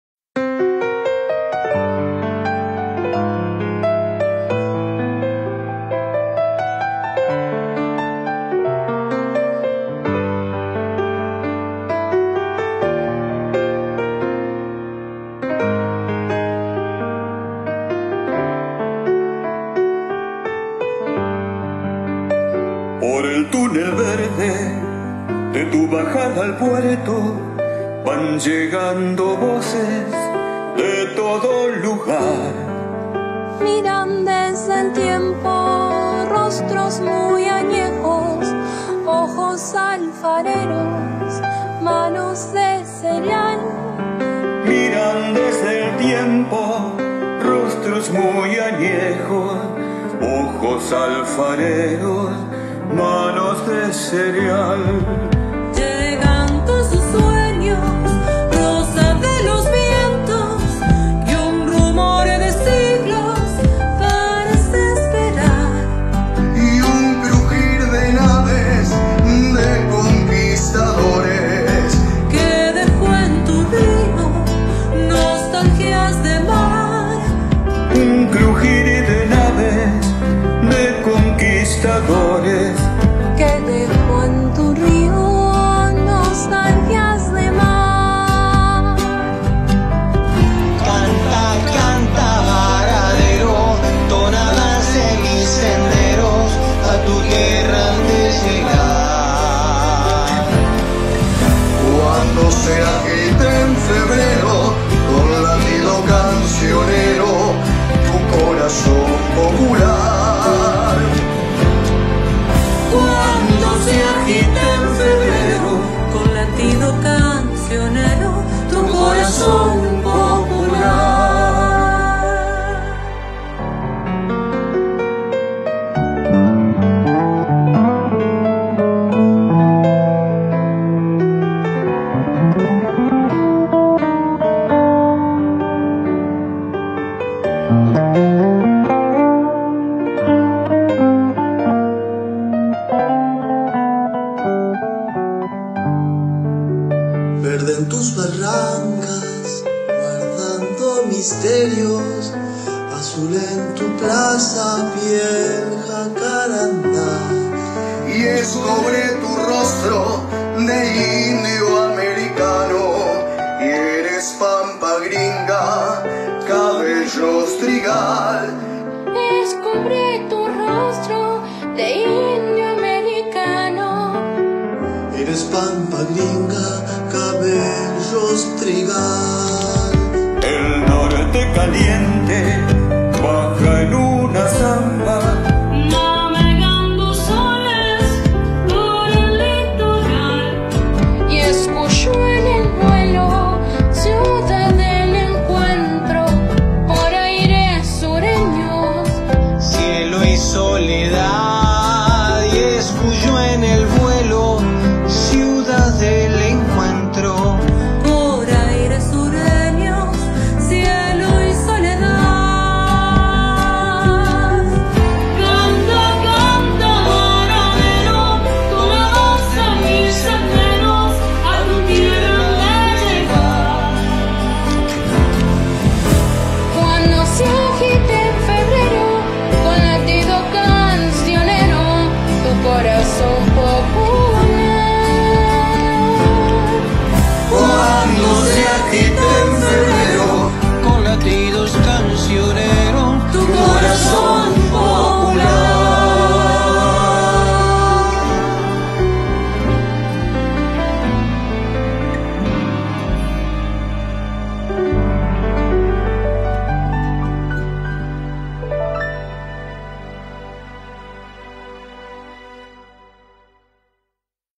CANTA BARADERO UNIDO
Jorge Romero y su apasionada canción convocaron a varios músicos locales 🎹🥁para que CantemosTodos 🎧🎤 en esta nueva etapa del aislamiento.

Nuestra ciudad cuenta con muchisimos grandes artistas que unidos pueden crear otras propuestas 🎵🎺🎸🎚para que CantemosEnCasa🏠